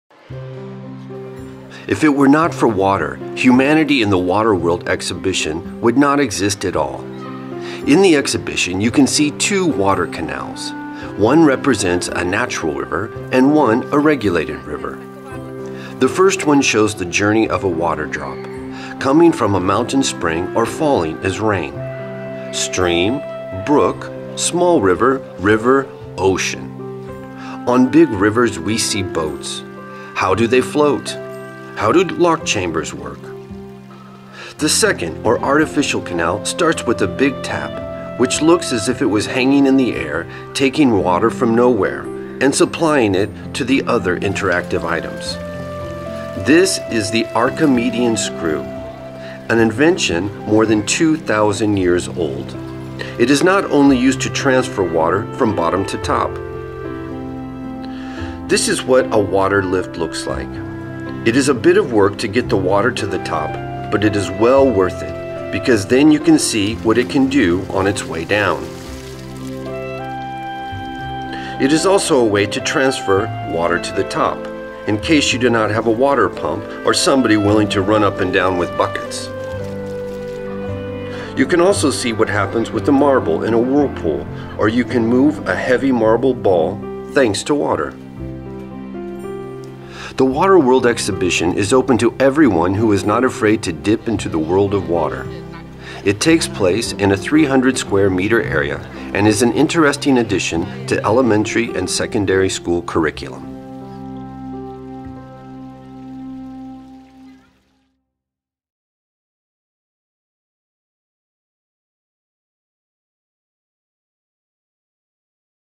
Techmania Science Center: Exhibition Water World
The Water World Exhibition consists of two model canals - artificial and natural.

Visitors may test how the natural water cycle works, from formation of precipitation and groundwater to its journey to the sea. The exhibition includes boats that visitors must get through the floodgates or the visitors can spin the mill wheel. The artificial canal offers, among other things, exhibits related to transportation of water – a model of Archimedes' screw, rotating scoops and a water lift are available to help students better understand the patterns of fluid mechanics. Thanks to this exhibition, people have the possibility to get sunk into learning natural laws.